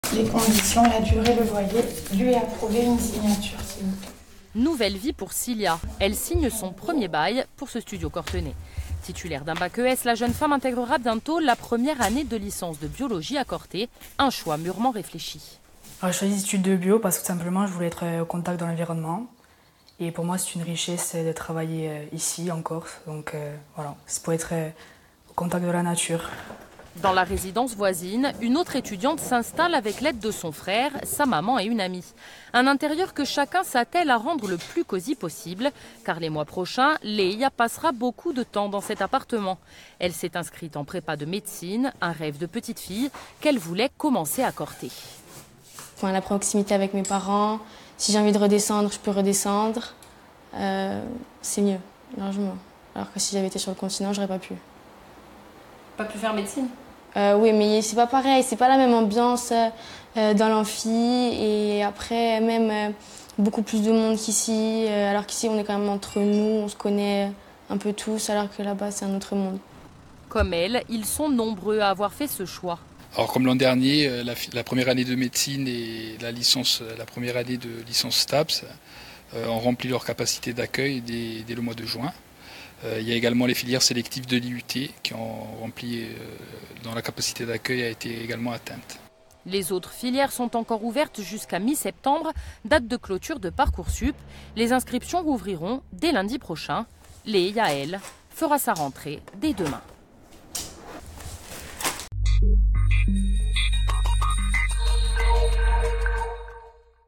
Université de Corse : à Corte, les étudiants commencent à s’installer
La rentrée se prépare. À Corte, l'Université rouvrira ses portes lundi 26 août, des étudiants commencent leur installation.

Intervenants - Cylia Le Corre, Etudiante en 1ère année de biologie ; Leia Finidori, Etudiante en 1ère année de médecine ; Dominique Federici, Vice-président de l'Université de Corse.
Equipe - Maia Graziani ; Lisa Macineiras ; Ramsey Kinany.